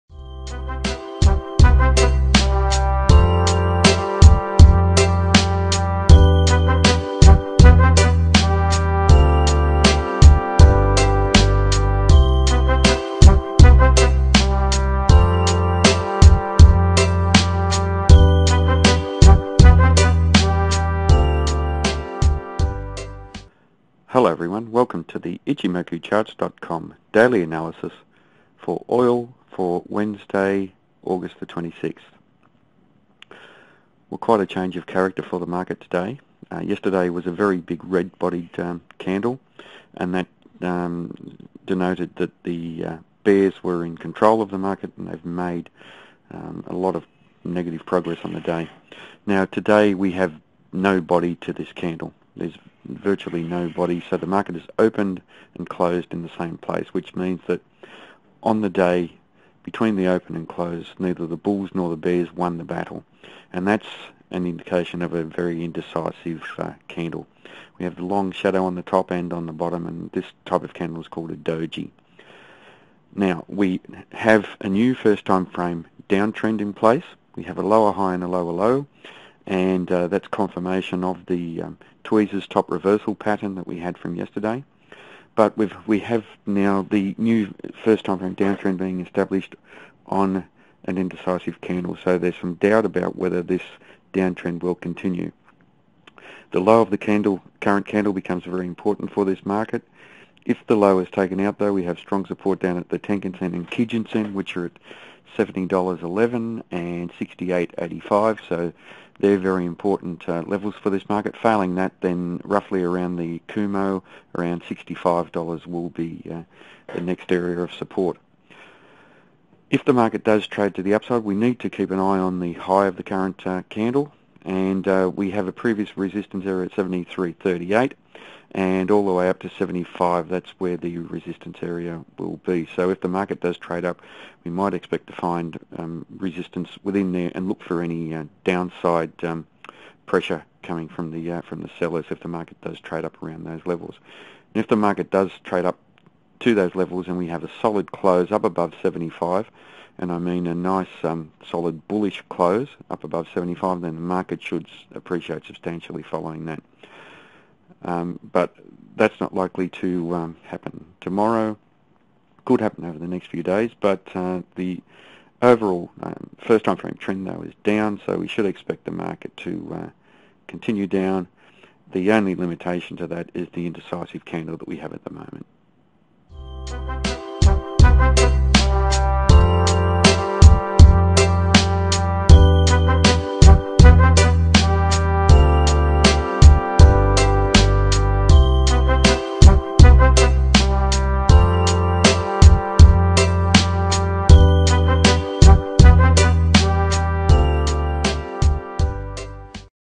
OilAug26th2009
Daily Oil analysis using Ichimoku charts for August 26th 2009